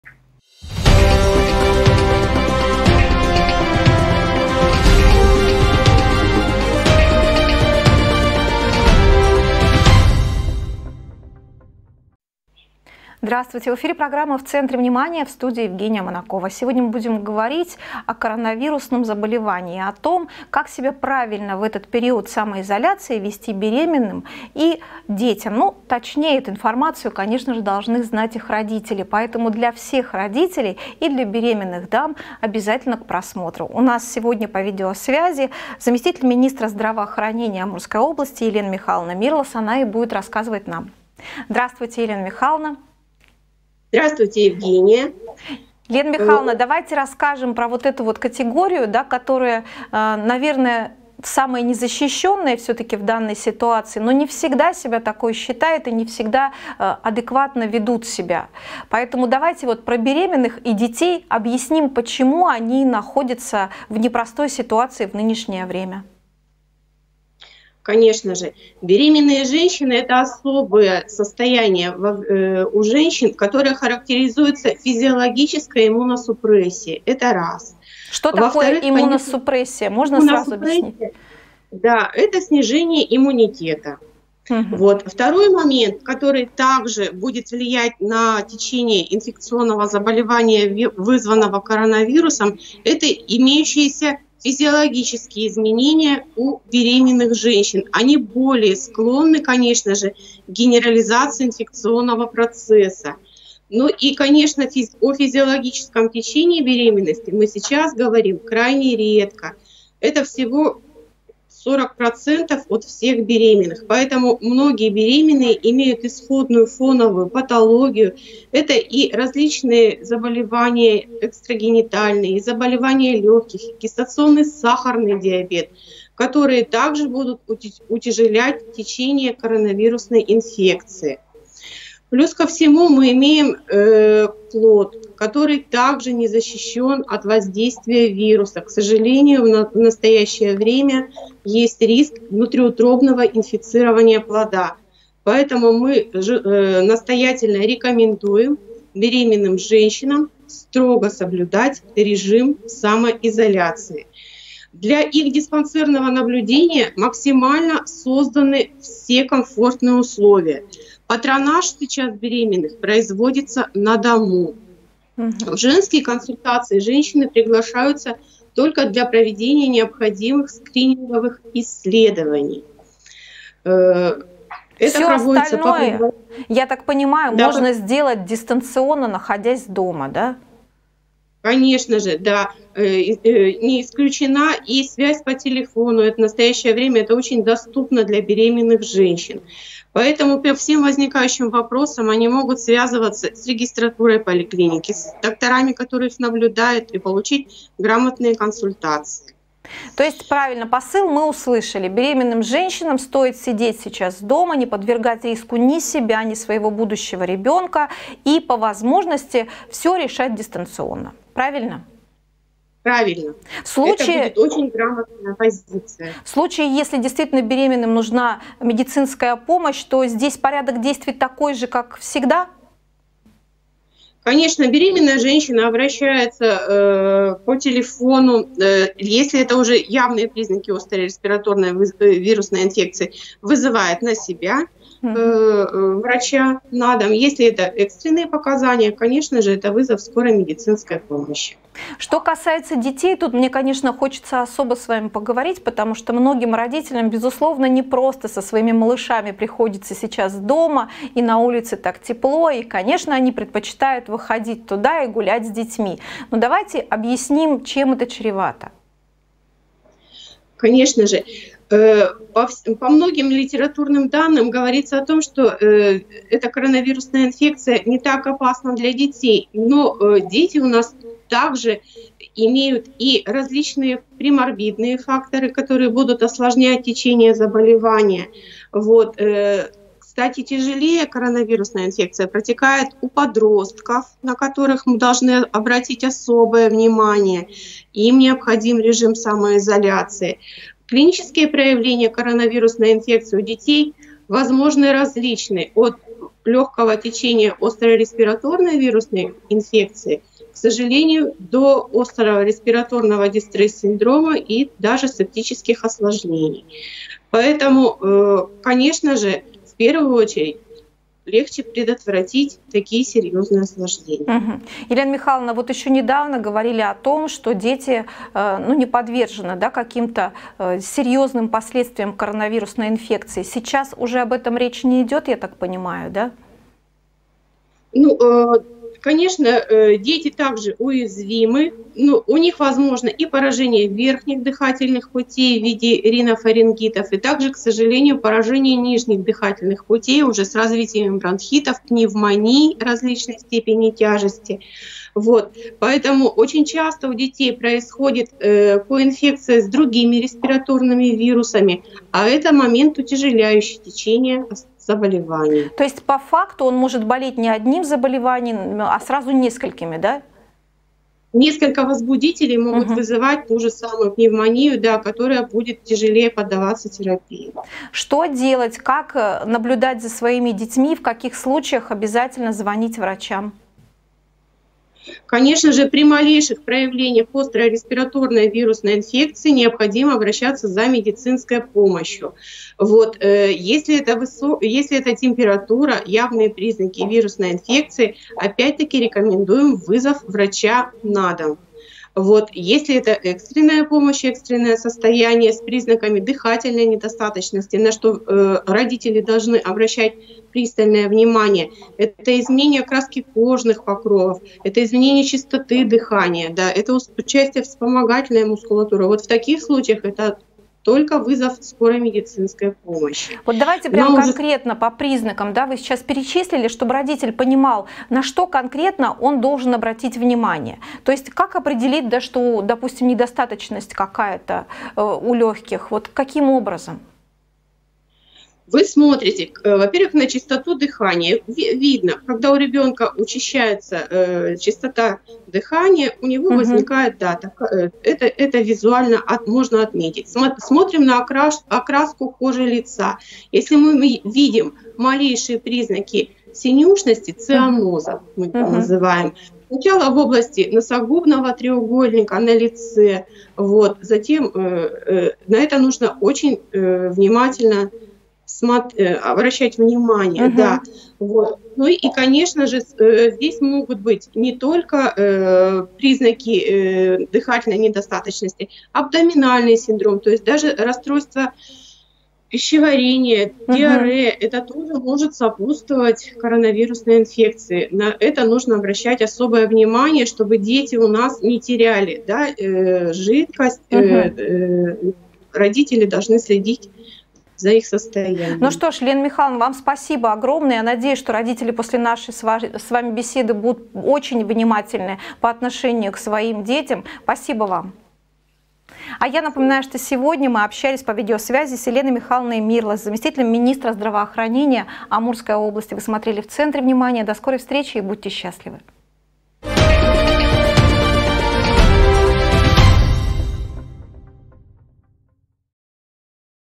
В центре внимания: Елена Мирлас
Заместитель министра здравоохранения Амурской области